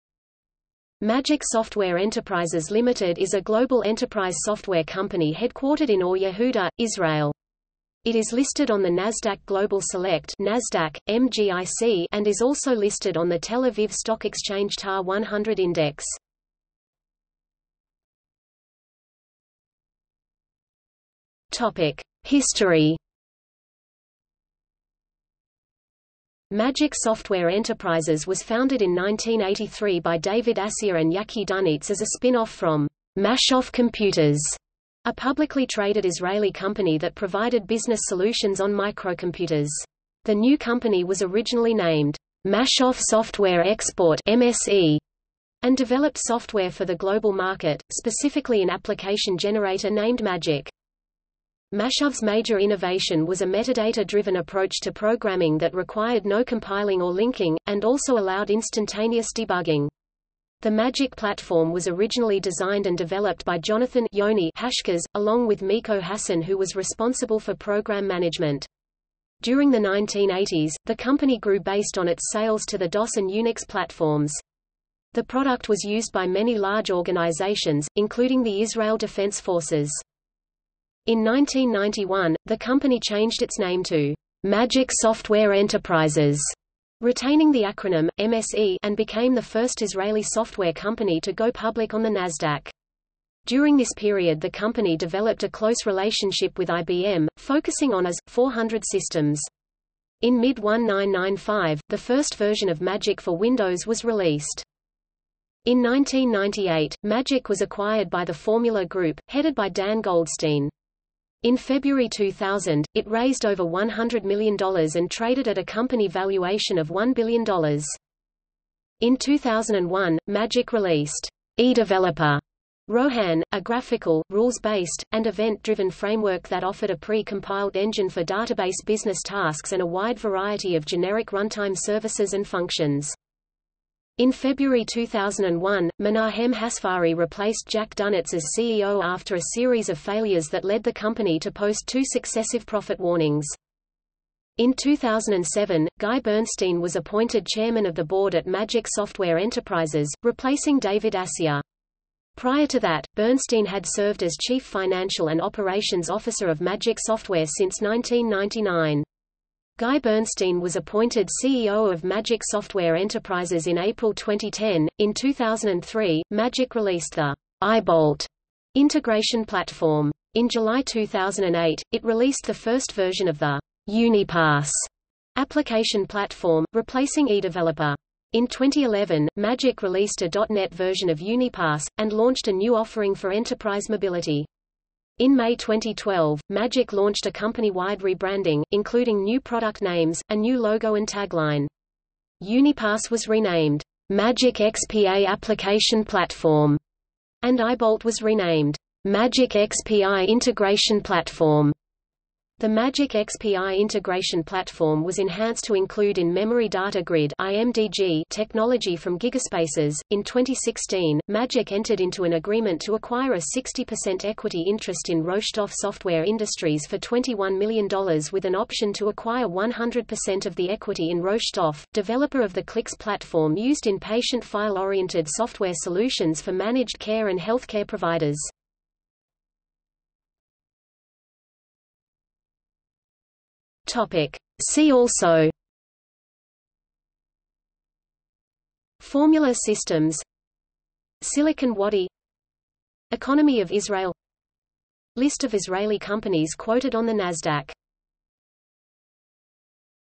Magic Software Enterprises | Wikipedia audio article
00:00:18 History
00:03:50 See also

- Socrates

SUMMARY
Magic Software Enterprises Ltd is a global enterprise software company headquartered in Or Yehuda, Israel. It is listed on the NASDAQ Global Select (NASDAQ: MGIC) and is also listed on the Tel Aviv Stock Exchange TA-100 Index.